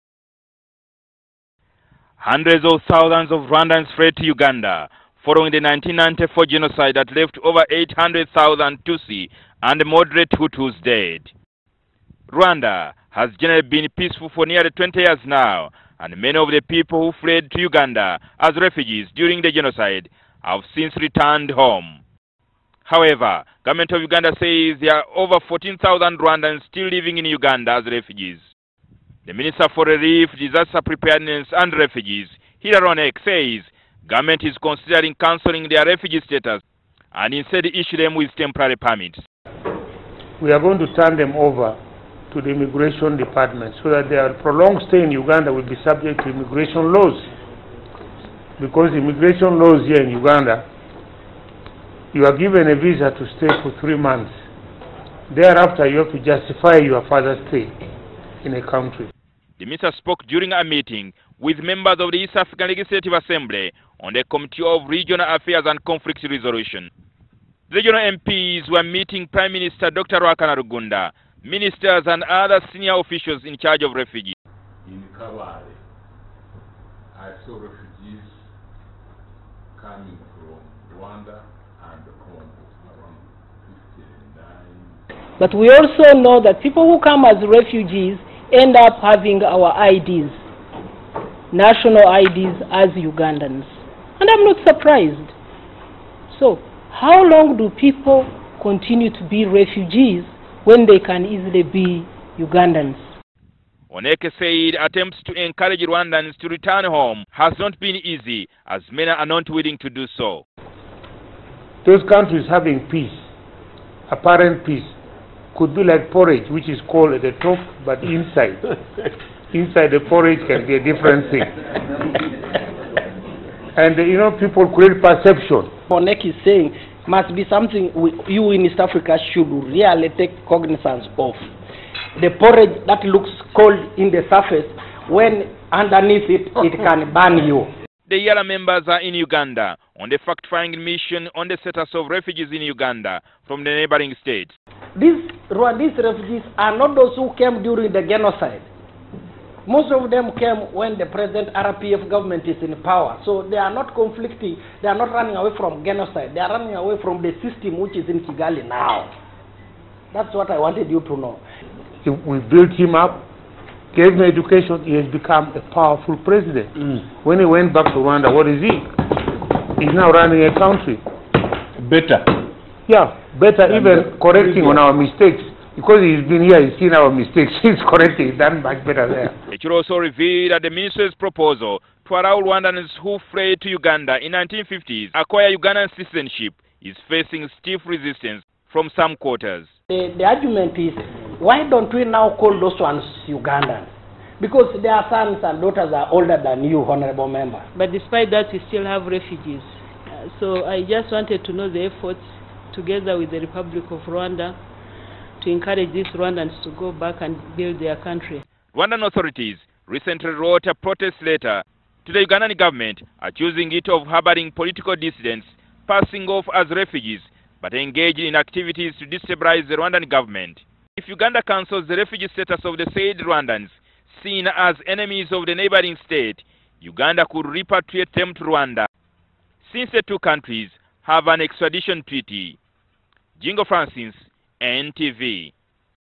Rwandan refugees in Uganda may be thrown out - Onek
The government of Uganda is considering cancelling the refugee status of thousands of Rwandans living in Uganda.

That is according to the Minister for Relief, Disaster Preparedness and Refugees Hillary Onek.

He made the announcement while meeting lawmakers of the East African Legislative Assembly in Kampala.

Onek said the Rwandans had no strong justification to stay in Uganda as refugees since Rwanda is peaceful.

He, however said government could review their status and issue them temporary permits through the Directorate of Citizenship and Immigration Control.